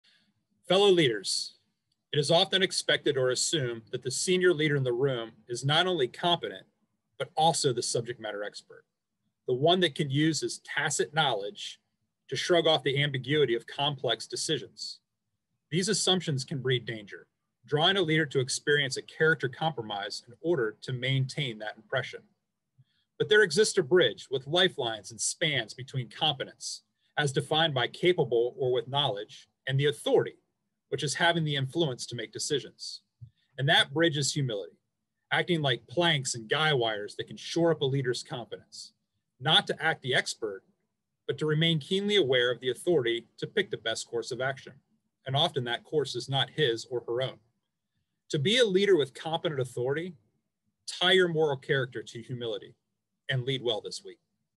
"45 Seconds on Leadership and Ethics" with U.S. Navy Capt. Hunter Bankart, military professor
Week 87 -- Leading with competent authority.